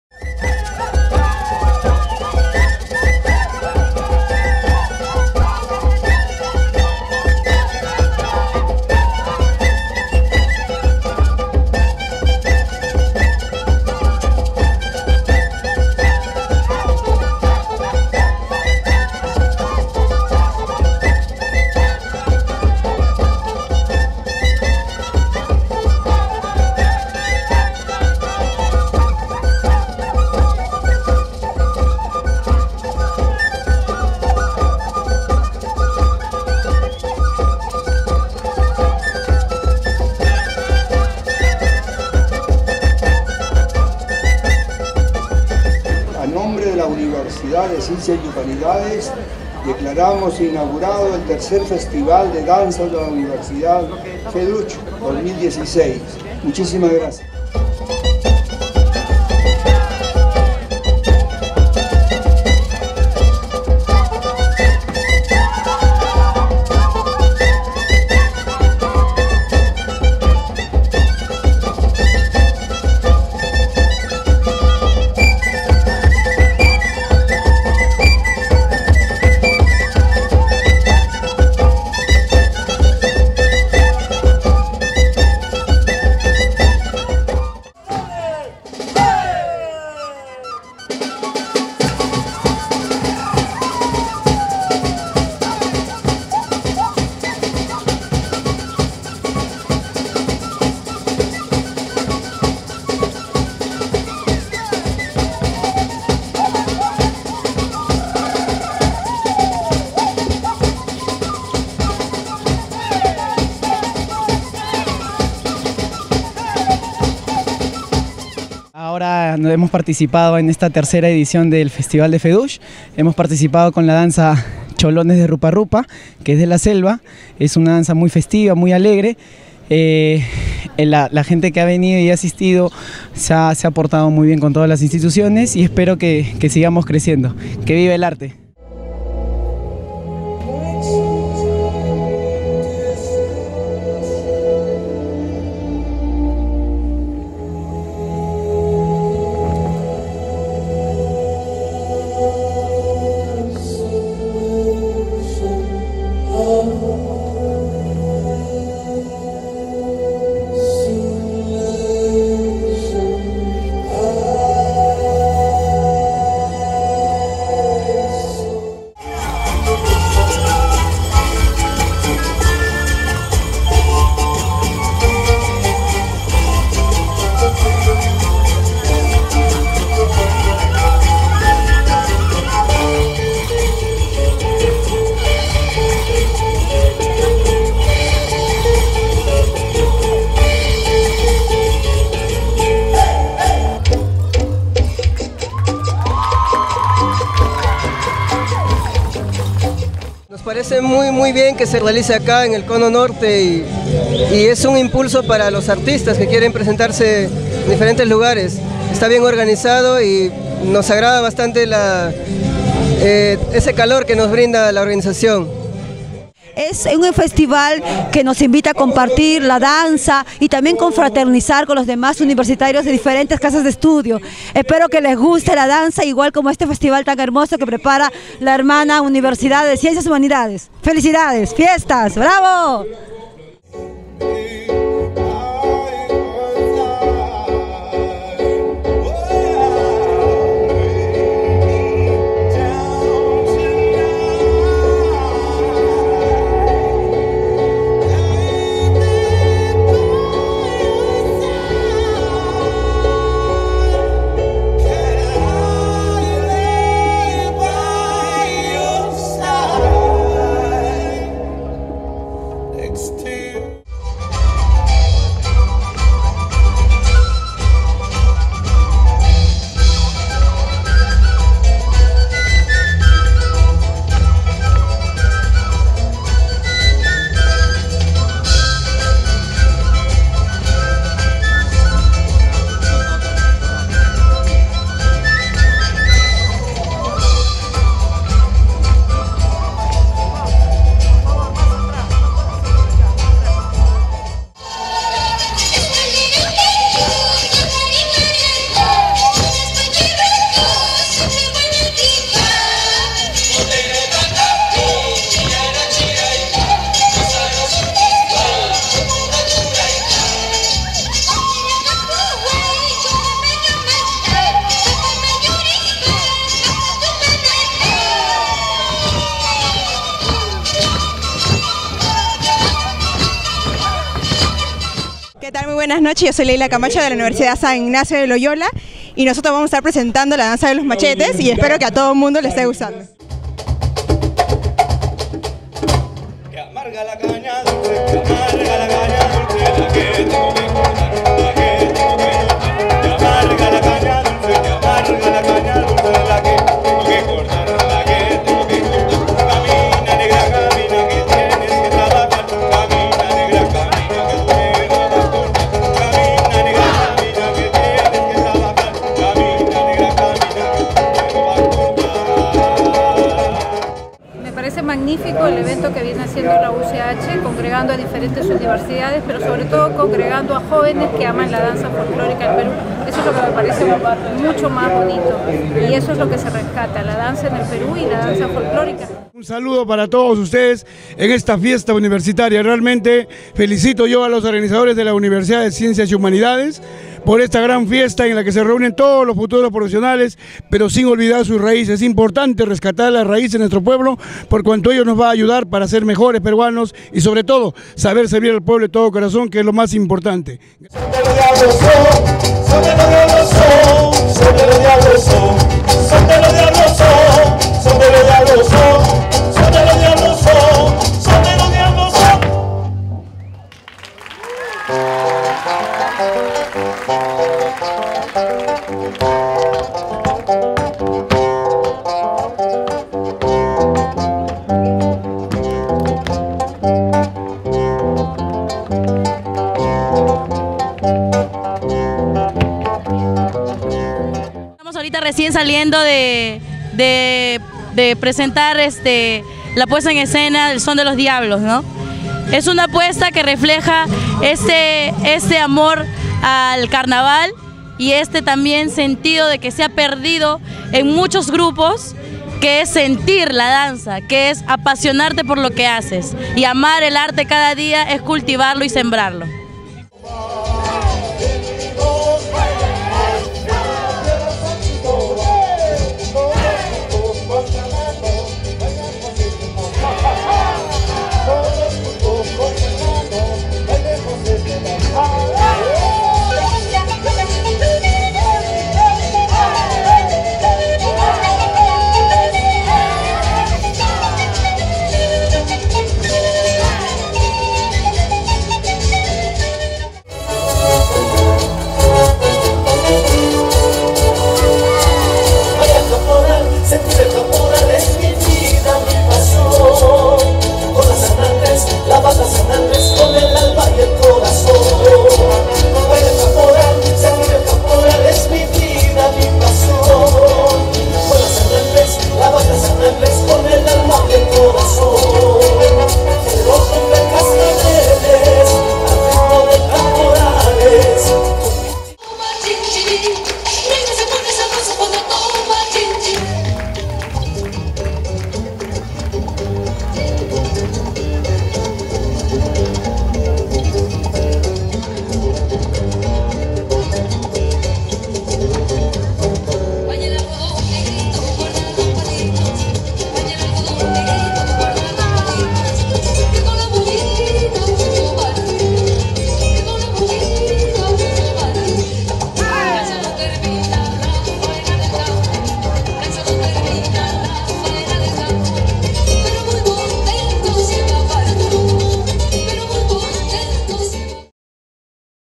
III Feduch - Viernes 16 de setiembre
III Festival de Danzas Universitarias FEDUCH.
Organizado por el Centro Cultural de la Universidad de Ciencias y Humanidades.